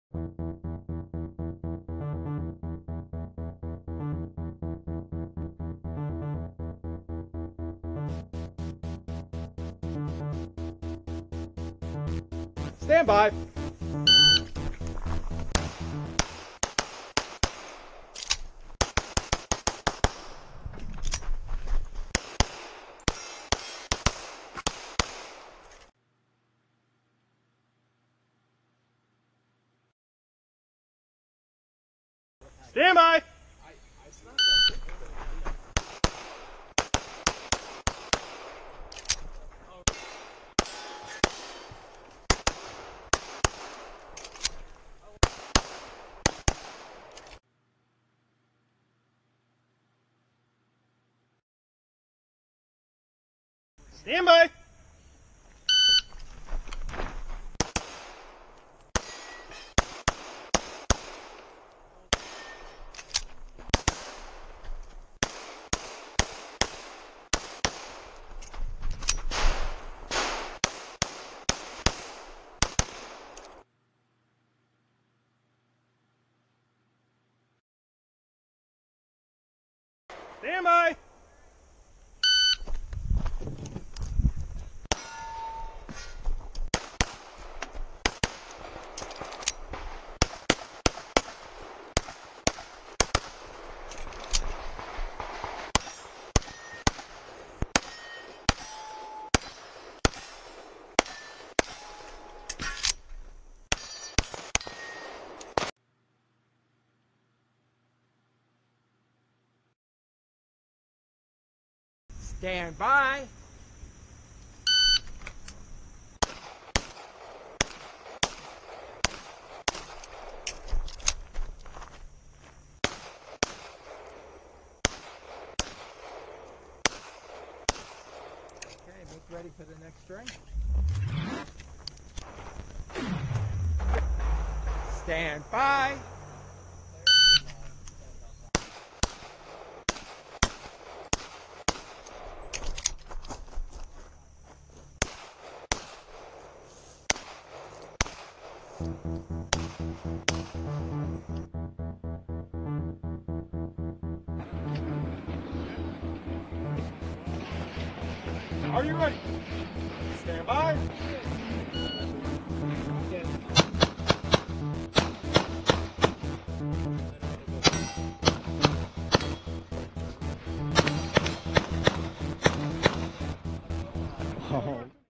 USPSA @ East Huntingdon - June 2022 - Single Stack Master
Division:  Single Stack
Rank (Single Stack):  1 of 2
Rank (Overall):  18 of 92
Gun:  Sig Sauer 1911 MAX
Ammo:  40 S&W (major)
Powder:  W231
Bullets:  SNS Casting 175gr SWC

0:00 Intro and Stats
0:10 Stage 3: Really, Really Fast
0:30 Stage 4: The Funnel
0:51 Stage 5: Angles And Leans
1:17 Stage 1: Paper And Steel
1:50 Stage 2: CM 13-03 "Short Sprint Standards"
2:34 Outro